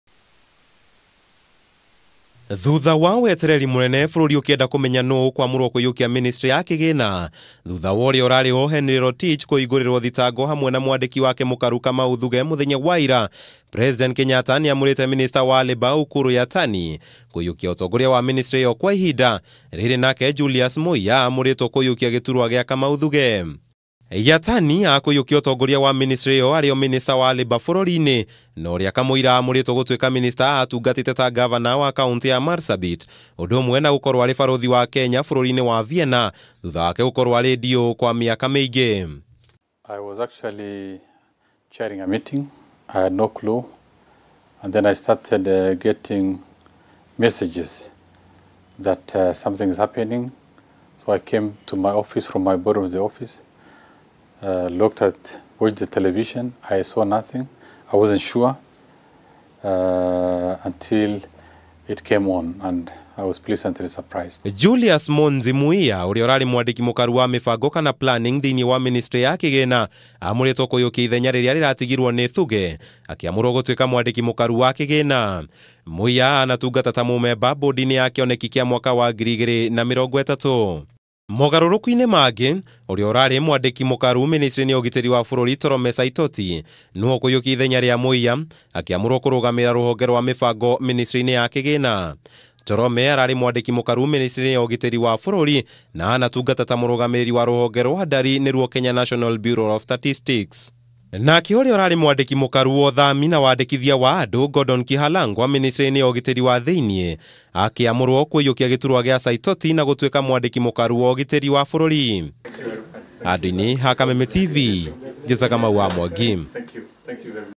Ukur Yattani kwamurwo ta minister wa ihinda ministry-ini ya kigina
Mutongoria wa bururi Uhuru Kenyatta niekite mogaruruku ndundu-ini yake ya maminista muthenya umwe tu thutha wa minista wa kigina Henry Rotich na mwandiki mukaru ministry-ini ino Kamau Thugge kuhingurirwo thitango ciegemanine na ngukimio ya ndemu cia Arror na Kimwarer
Hari ugaruruku uyu minister wa Labour Ukur Yattani niwe ukunyitirira kwa ihinda ithenya ria rotich nake Julius Muiya uria urari mwandiki mukaru ruhonge-ini rwa mibango akioya ithenya ria Kamau Thugge
Torome Saitoti uria urari mwandiki mukaru ministry-ini ya ugitiri wa bururi ekuiyukia giturwa kia muiya ari ta mwandiki mukaru wa mibango.